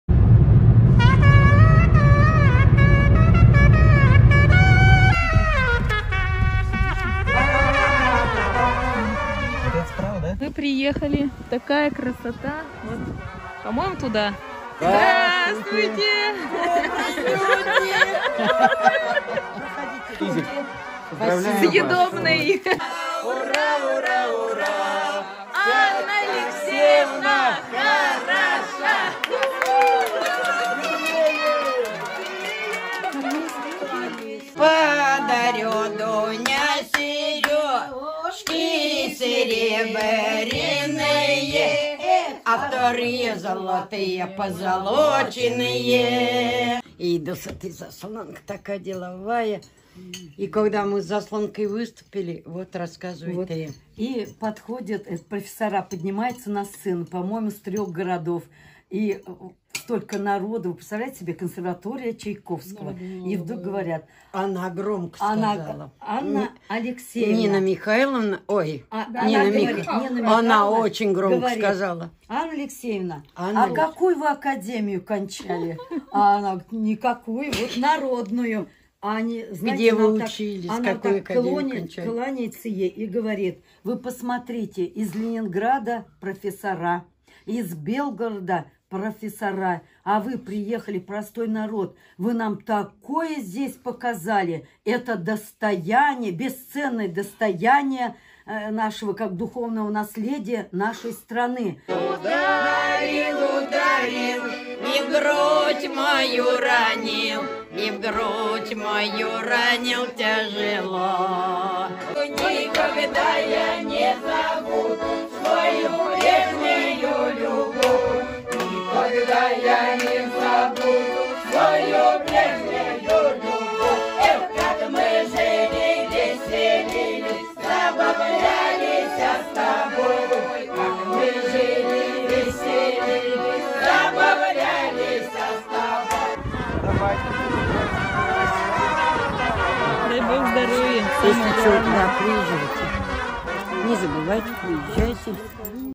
85 лет А. А. Ефремовой, село Карадули Лаишевского района. Видео от ансамбля "Верес" (Казань)
Татарстан, Россия.